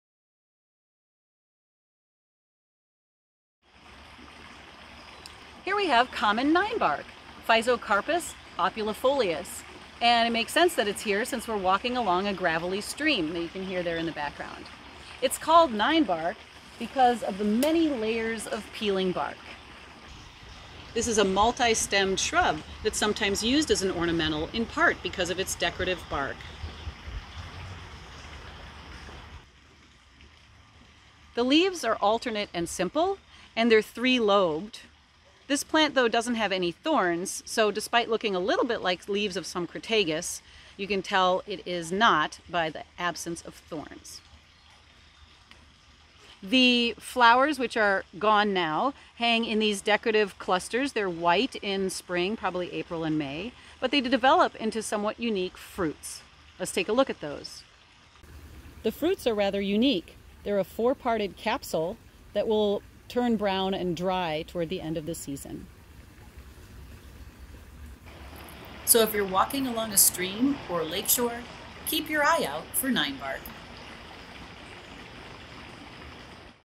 Physocarpus opulifolius - ninebark
Dr. Catherine Woodward explains the botanical characters for identifying ninebark.